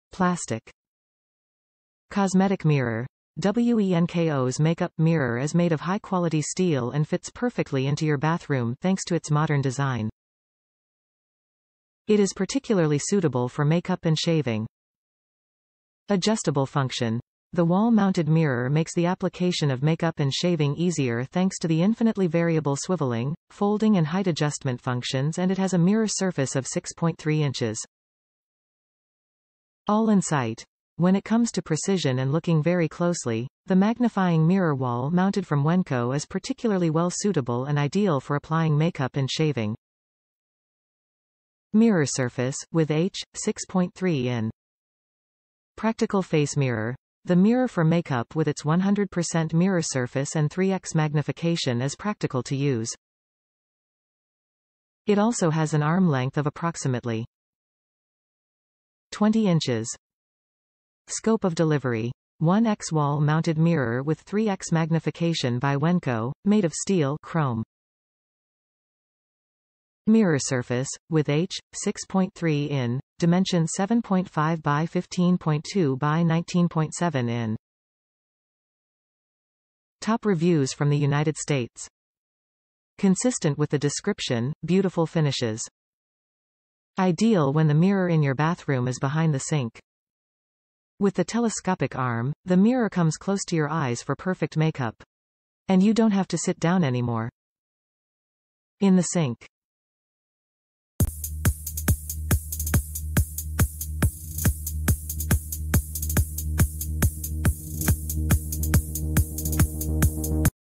Wenko, Chrome, Wall Mounted Bathroom, Magnifying Makeup Mirror For Cosmetic Use, Steel, Dimensions
Plastic
COSMETIC MIRROR: WENKO's make up mirror is made of high quality steel and fits perfectly into your bathroom thanks to its modern design. It is particularly suitable for make-up and shaving
ADJUSTABLE FUNCTION: The wall mounted mirror makes the application of make up and shaving easier thanks to the Infinitely variable swivelling, folding and height-adjustment functions and it has a mirror surface of 6.3 inches
ALL IN SIGHT: When it comes to precision and looking very closely, the magnifying mirror wall mounted from WENKO is particularly well suitable and ideal for applying makeup and shaving. Mirror surface (W/H): 6.3 in
PRACTICAL FACE MIRROR: The mirror for makeup with its 100% mirror surface and 3x magnification is practical to use. It also has an arm length of approx. 20 inches
SCOPE OF DELIVERY: 1x wall mounted mirror with 3x magnification by WENKO, made of steel, chrome. Mirror surface (W/H): 6.3 in, dimensions 7.5 x 15.2 x 19.7 in
 Consistent with the description, beautiful finishes
ideal when the mirror in your bathroom is behind the sink. With the telescopic arm, the mirror comes close to your eyes for perfect makeup! And you don't have to sit down anymore... in the sink